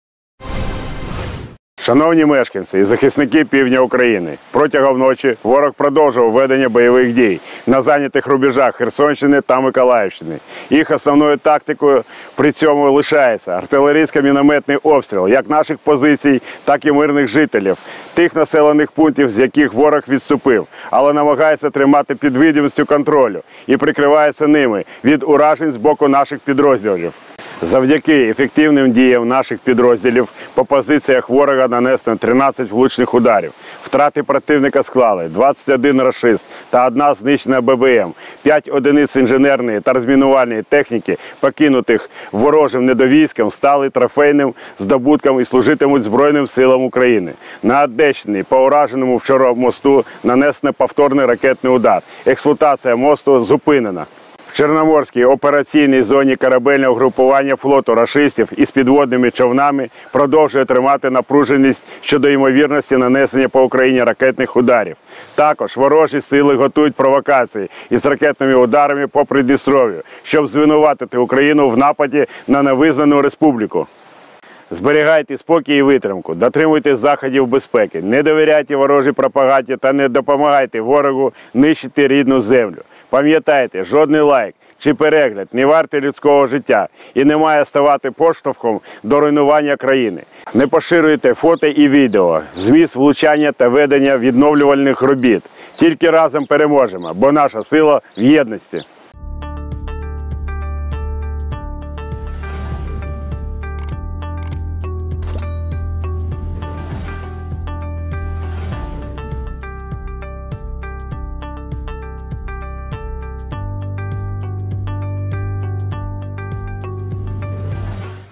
13 раз влупили по оркам: ВСУ на Юге взяли 5 трофеев тяжелой техники и уничтожили десятки рашистов
Оперативная информация по ситуации на юге Украины в 63-й день войны:

Потери противника составили 21 рашист и 1 уничтожено ББМ. А 5 единиц инженерной и разминочной техники покинутых вражеским недовойском стали трофейным достижением и будут служить Вооруженным Силам Украины.